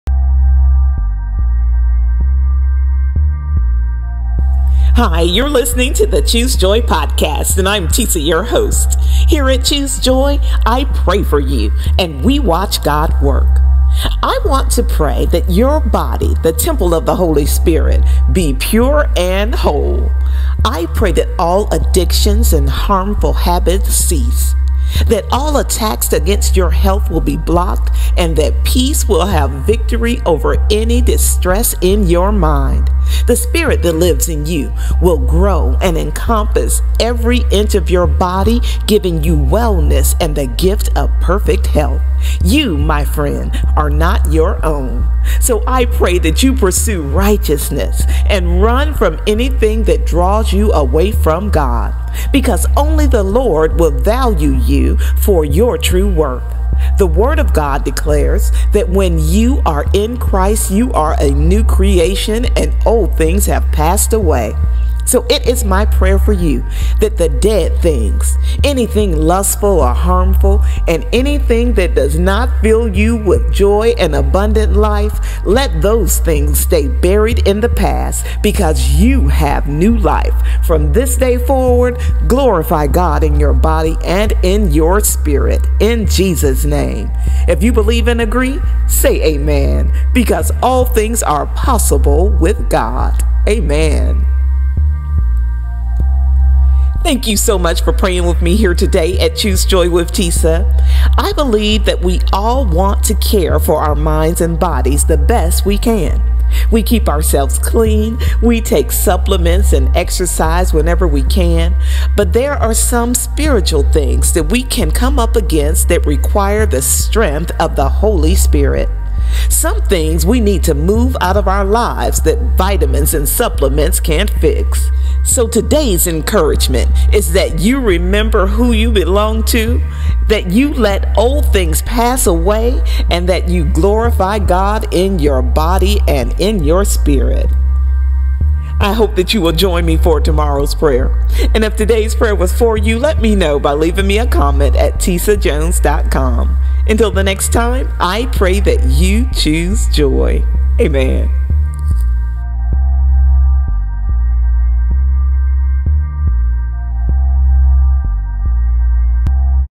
812 Your Body is the Temple of the Holy Spirit | Choose Joy with Tisa
Choose Joy with Tisa podcast
Season 8
Episode 12

Your Body is a Temple of The Holy Spirit
Or do you not know that your body is the temple of the Holy Spirit who  is in you, whom you have from God, and you are not your own? For you were bought at a price; therefore glorify God in your body and in your spirit, which are God’s. I Corinthians 6:19‭-‬20 NKJV‬‬

Therefore, if anyone is in Christ, he  is  a new creation; old things have passed away; behold, all things have become new. II Corinthians 5:17 NKJV

Flee also youthful lusts; but pursue righteousness, faith, love, peace with those who call on the Lord out of a pure heart. II Timothy 2:22 NKJV